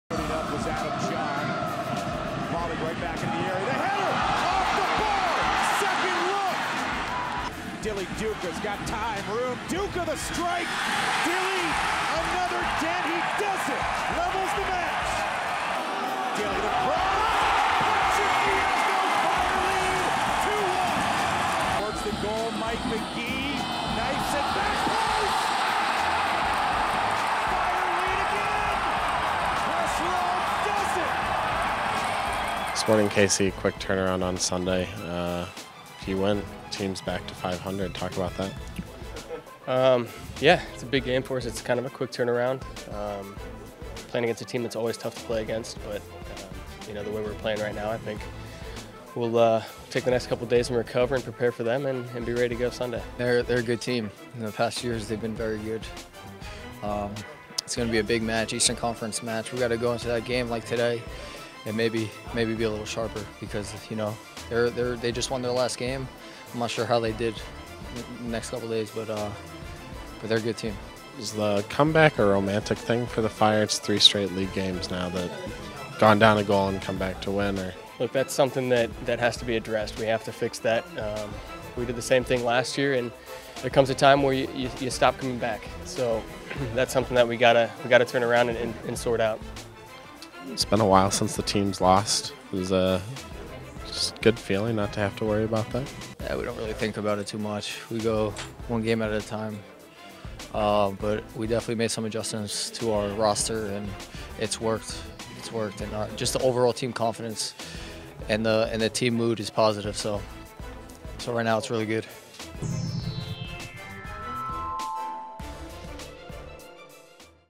Match Preview | Chicago Fire vs. Sporting KC
Forward Chris Rolfe, and Midfielder Dilly Duka comment on the Fire's upcoming match up vs. rival Sporting KC this Sunday @ Toyota Park.